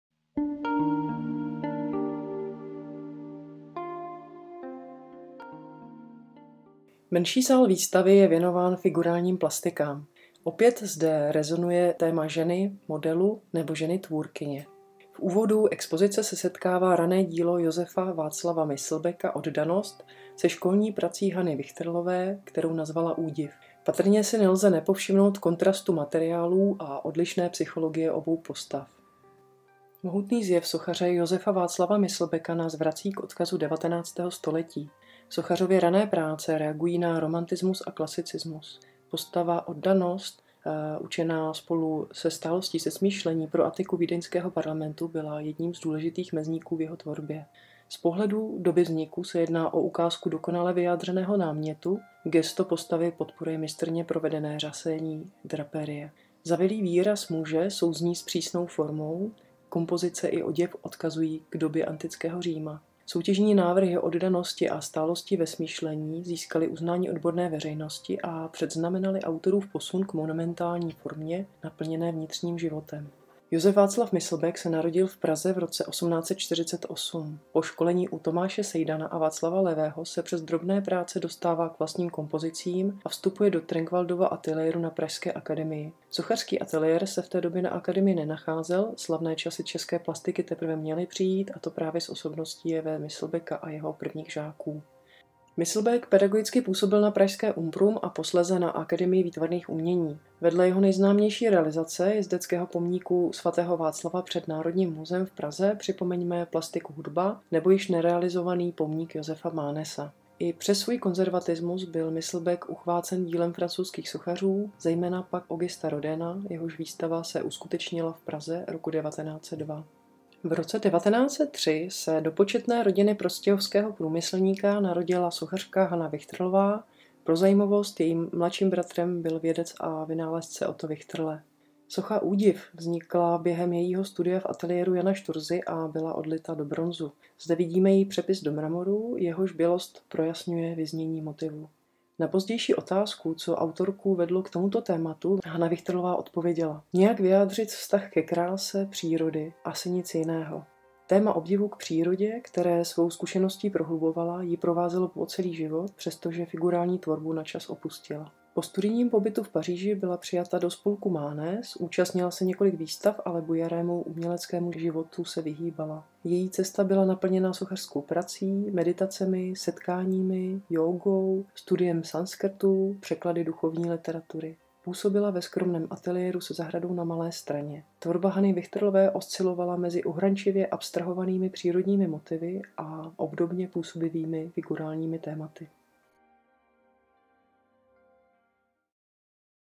Sochy mluví... medailon z výstavy VIII || Josef Václav Myslbek + Hana Wichterlová-Stefanová
Josef Václav Myslbek, Oddanost + Hana Wichterlová-Stefanová, Údiv
V krátkých medailonech představujeme autory a díla z výstavy Sochy mluví ze sbírek Oblastní galerie Vysočiny v Jihlavě.
Komentář připravila a namluvila kurátorka výstavy Ilona Staňková.